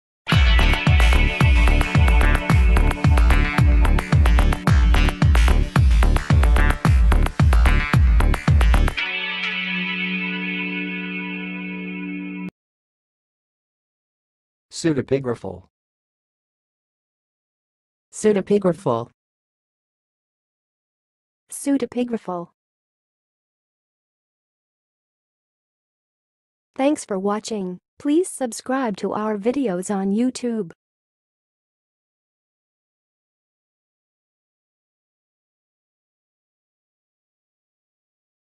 How to say "pseudepigraphal"! (High Quality Voices)
Watch in this video how to say and pronounce "pseudepigraphal"!